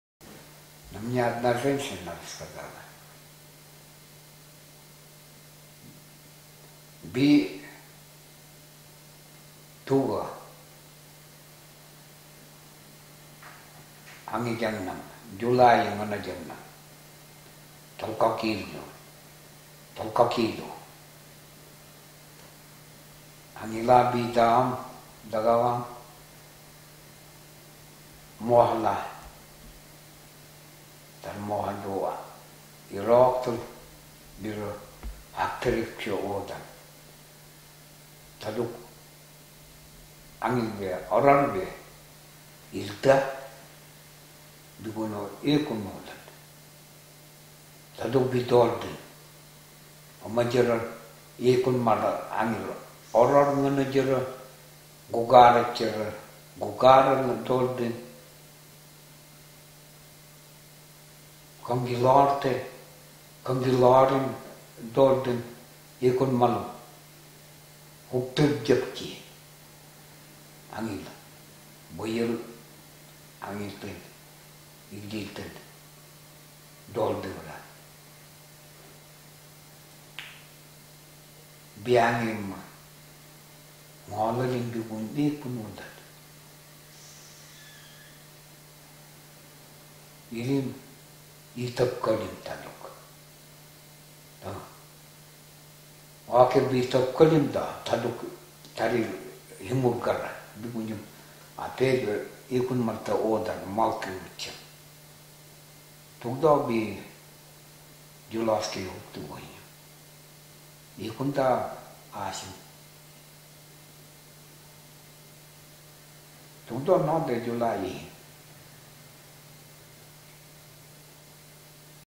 Зимой в лесу (Г. К. Лапуко, пос. Тура)
Текст записан в 2008 году в Туре. Геннадий Константинович Лапуко рассказывает на эвенкийском языке о случае, который произошел с одной женщиной. Она ехала одна на оленях и вдруг услышала, как кто-то приближается. Голоса, звуки колокольчиков привели ее в ужас. Кто это был?